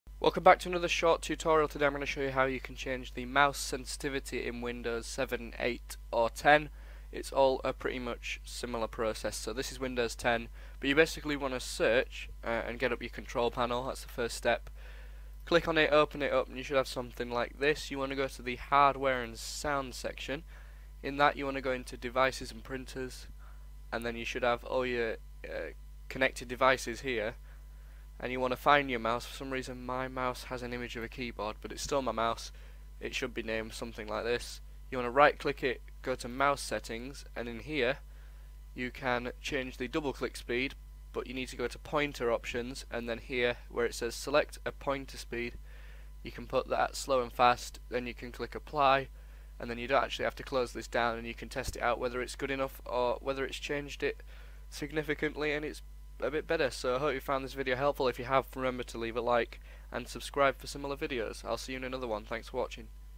How to Change Mouse Speed/Sensitivity in Windows 10
Mouse too fast/sensitive or too slow? In this video I'll show you how to change mouse speed/sensitivity in Windows 10. This fix also works in Windows 7, 8 and 8.1. If your mouse is too fast or too slow, changing the sensitivity could fix it. The fix is quite easy so hopefully the solution helps!